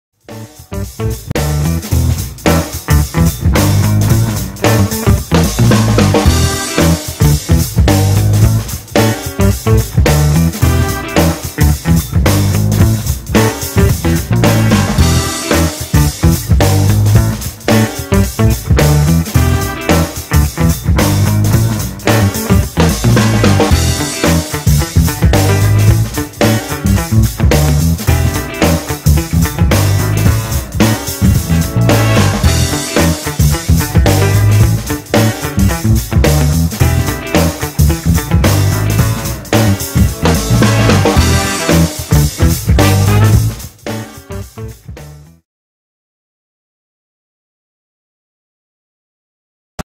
What is EMS training and why come to FITIN20 to get fit and healthy?
This video will give you an impression what it is like to work out at FITIN20. We are an EMS studio that uses German technology to improve the efficiency of a (low impact) workout by 18x. Just 20 minutes a week is enough to get amazing results. You work out with your own highly experienced and motivating Personal Trainer in a friendly and inviting environment. The PT will design your workout for your needs taking into account injuries, age and goals. Most people describe the feeling (especially in the back) as a tingly deep tissue massage, weird but pleasant!
Why not have the experience and come try it out for FREE?